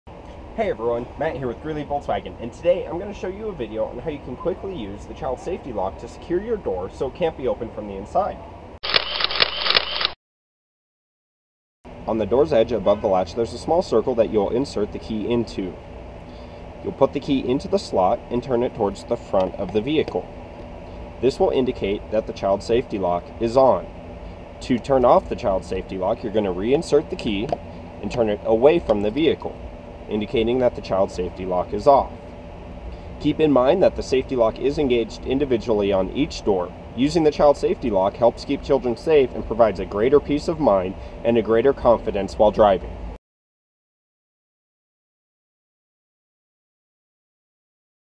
VOLKSWAGEN CHILD SAFETY LOCK
Hey everyone today i'm going to show you how to turn on the child safety lock on your Volkswagen.